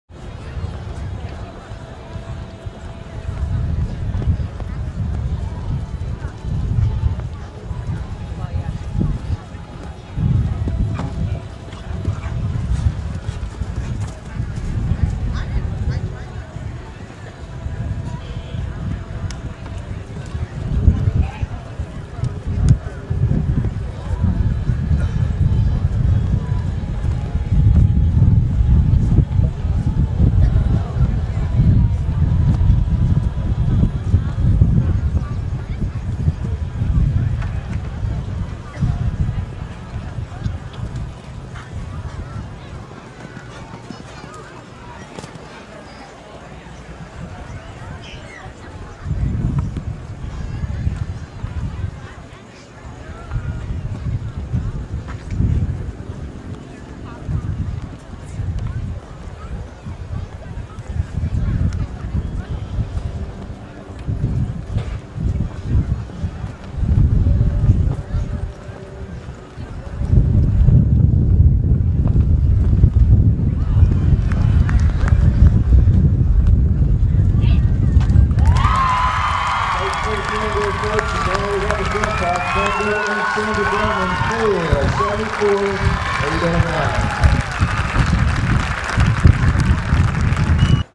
Tango Traders Point $60k Grand Prix 2011 4th place
Tango and Sandra Dalman finish 4th in the $60,000 Traders Point Grand Prix August 14th in Zionsville, IN. Sandra rides for Southview Stables out of Fort Wayne, IN. First Round Clear